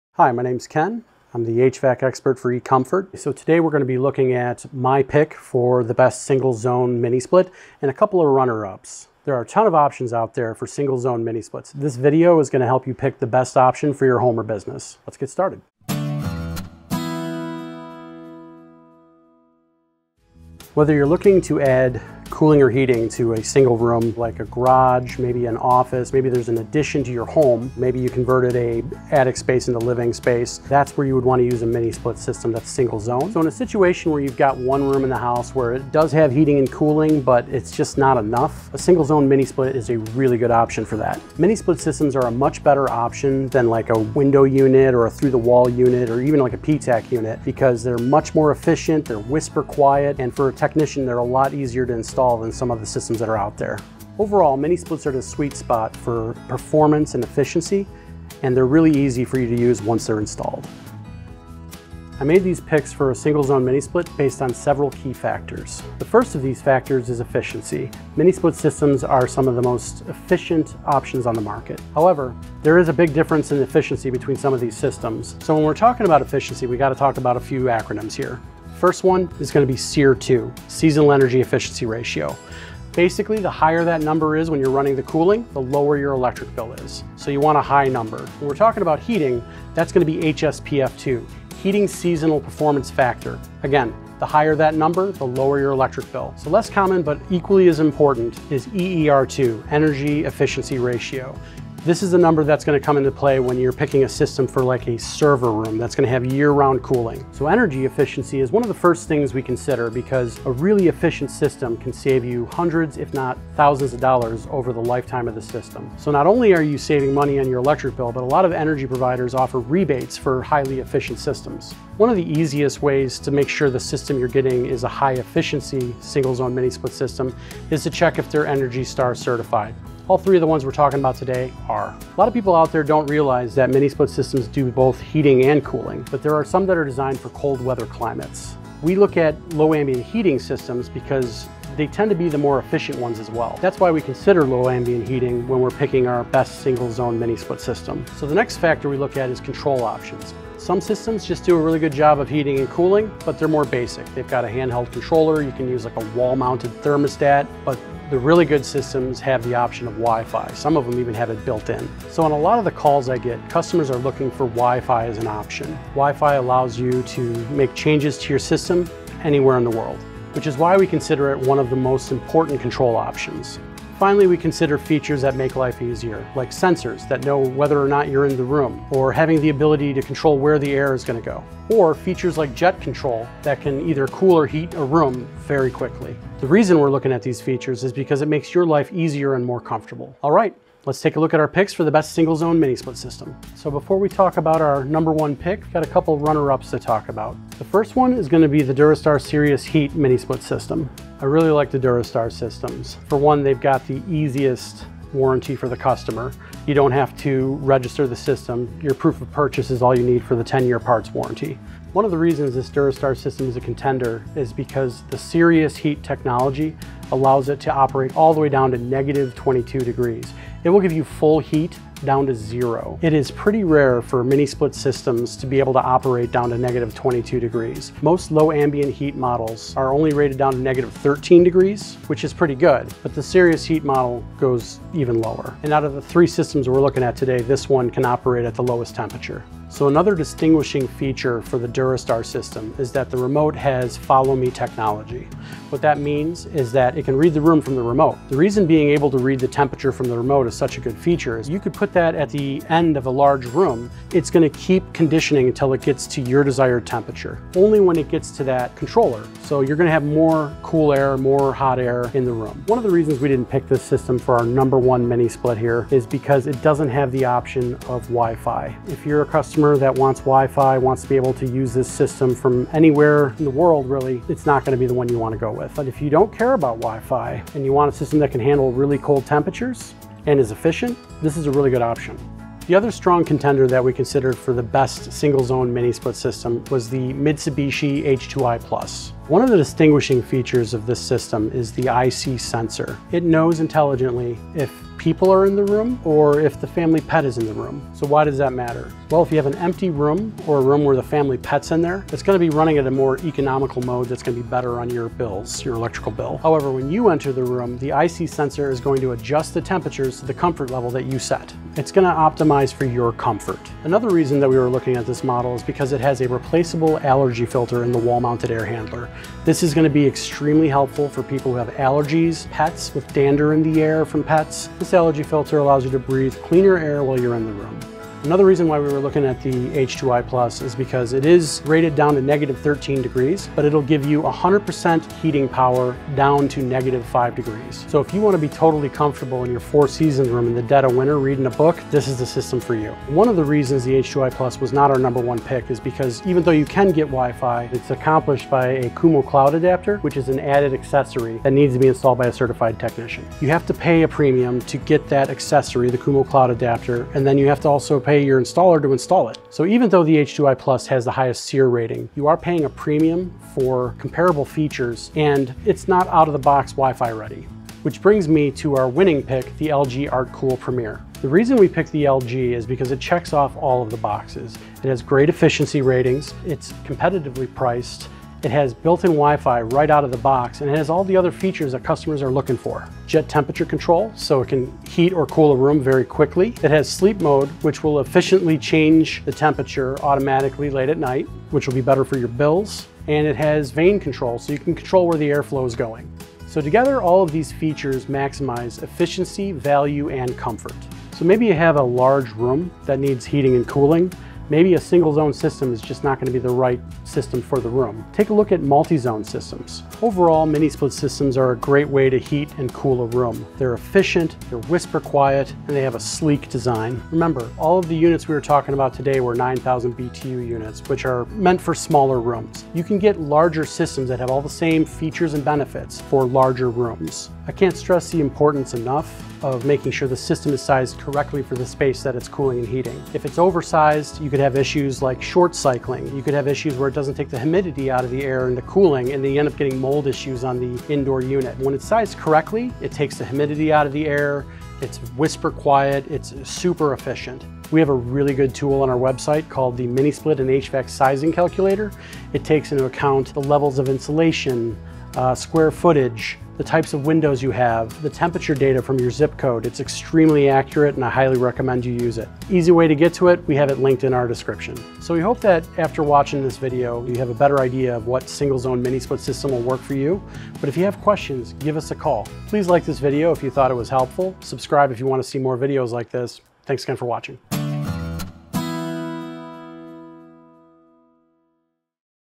The Best Single-Zone Mini Splits — Feature Comparison & Review
HVAC Expert Ken walks you through our picks for the best single-zone mini split. Ken walks you through our criteria and provides solid reasoning behind our #1 pick for the best single-zone mini split, as well as the two runner-up considerations.

Chapters:
0:00 Introduction
1:00 Criteria We Used to Make the Picks
1:15 SEER2, HSPF2, EER2 Ratings Explained
3:13 Runner Up Pick
3:22 Why We Love It
4:22 Why It Didn't Win
4:42 Next Runner Up Pick
4:53 Why We Love It
5:43 Why It Didn't Win
6:07 Our Winning Pick for Best Single-Zone Mini Split
6:16 Why It's Our #1 Pick
6:49 Wrap Up
7:27 Mini Split Sizing Calculator Overview
7:40: Outro
The product experts at eComfort & Power Equipment Direct answer your most common power equipment questions.